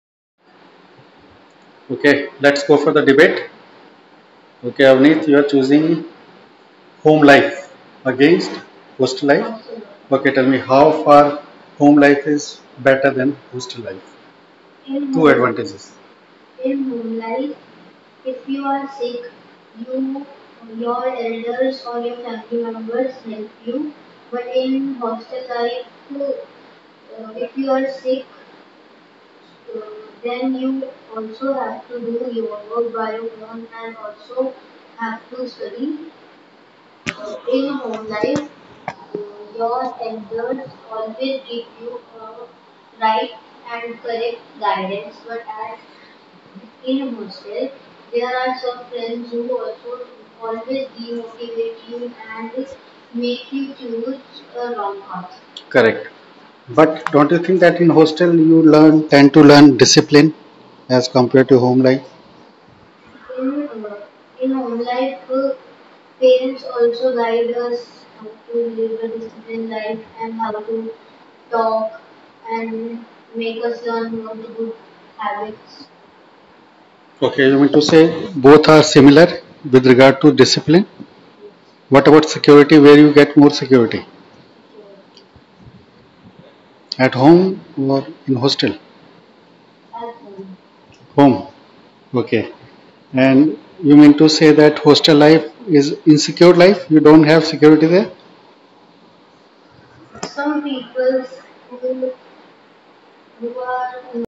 Home life offers several advantages over hostel life | Bafel | Spoken English in Dwarka
🏠 Home life offers several advantages over hostel life. Here are some of the reasons why home life can be more favorable:
💛 Comfort and Familiarity: Home provides a familiar and comfortable environment where individuals can truly relax and be themselves.
🍲 Home-Cooked Meals: Home-cooked meals are healthier, personalized, and often more delicious than hostel food.
🧡 Emotional Support: Family members offer emotional support and understanding during challenging times.
💤 Better Sleep Quality: Sleeping at home in a familiar bed can lead to better sleep quality and restful nights.
💸 Cost-Effective: Living at home can save money on accommodation and other expenses associated with hostel life.
💕 Family Bonding: Staying with family promotes stronger bonds and meaningful connections with loved ones.
🏠 Personal Space: Having one's personal space at home allows for privacy and individuality.
👪 Shared Responsibilities: At home, family members often share household chores, making daily tasks more manageable.
🍂 Childhood Memories: Home evokes nostalgic memories and a sense of belonging to one's roots.
👵👴 Supportive Elders: For younger individuals, living at home may provide the wisdom and guidance of grandparents or older family members.
📚 Study Environment: Home can offer a quieter and more focused study environment for students.
❤️ Unconditional Love: The love and care received at home create a sense of emotional security and stability.
Overall, home life provides a nurturing and comforting atmosphere, fostering personal growth and well-being. 🏠